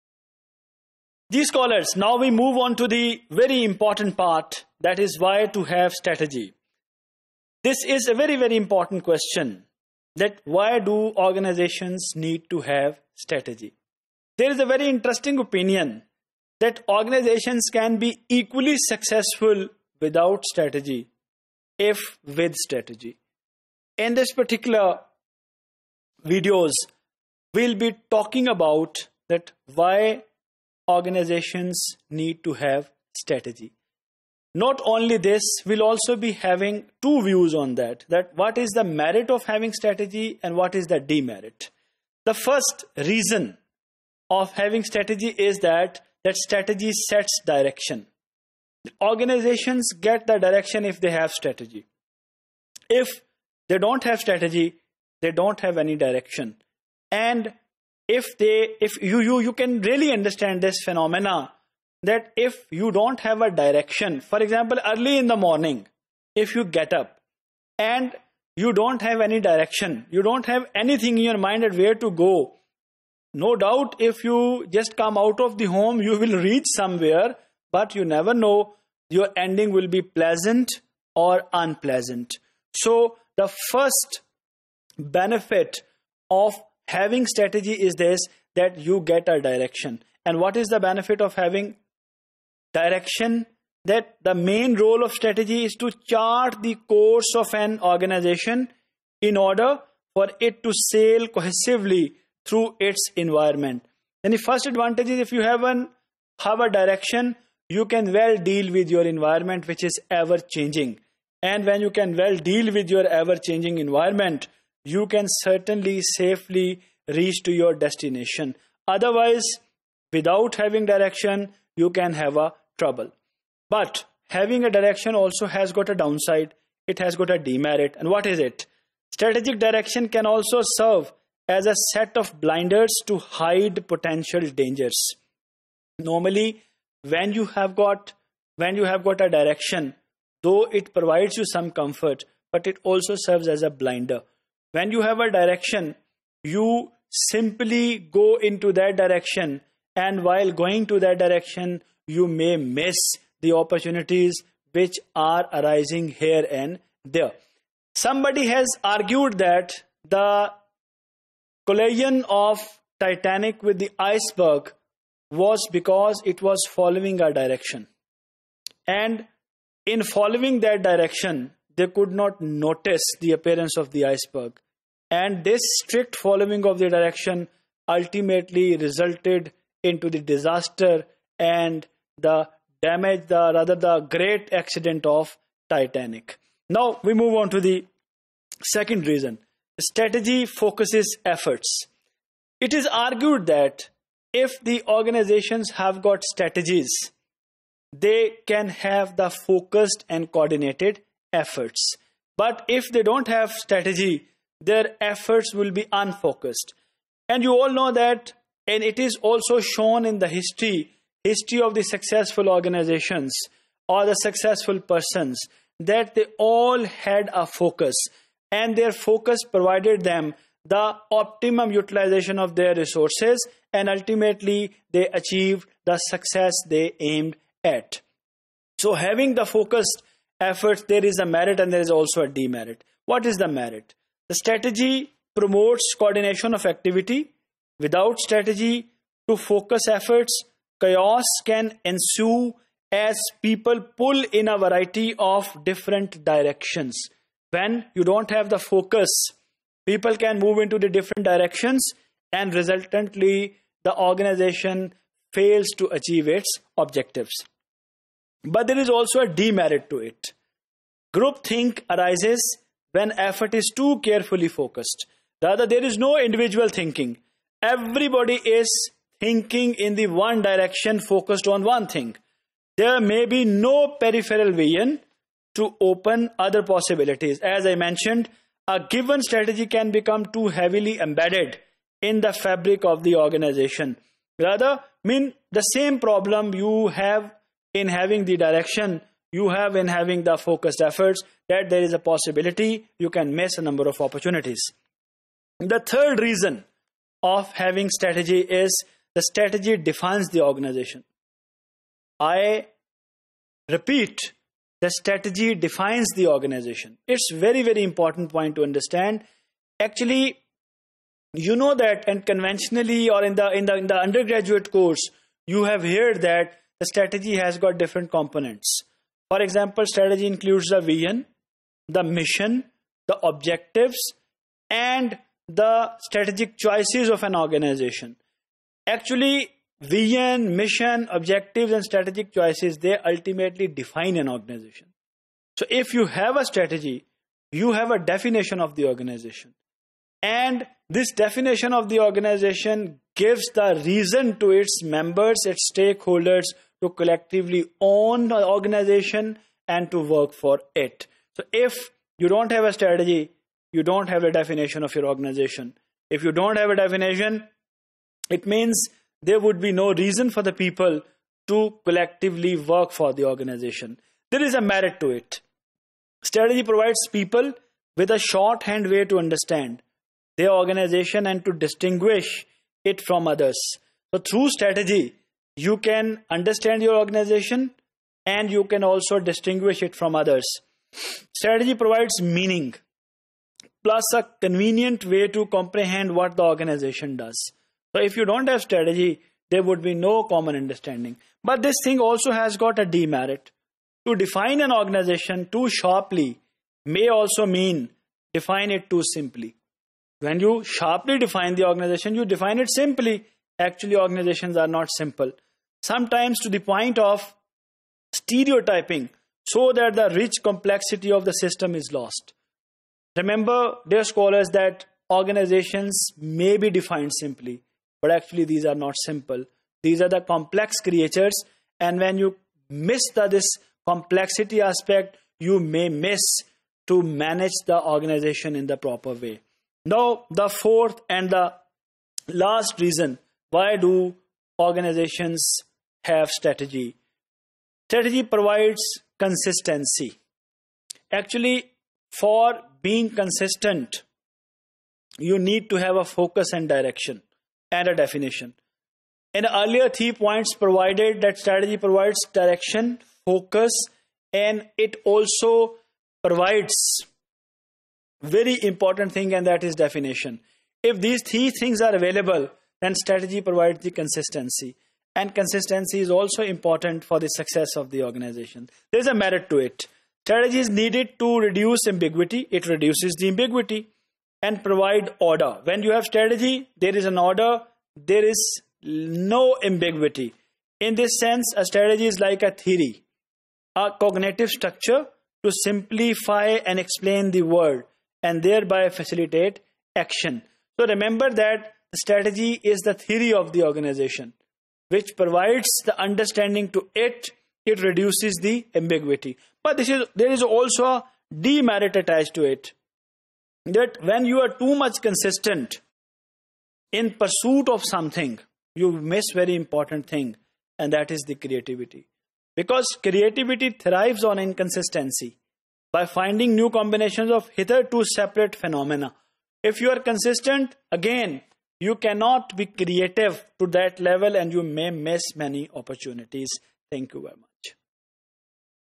Why to Have Strategy ? | Strategic Management | MGT703_Topic005
MGT703 - Strategic Management
Topic: 05 Why to Have Strategy
Mr. Rizwan Saleem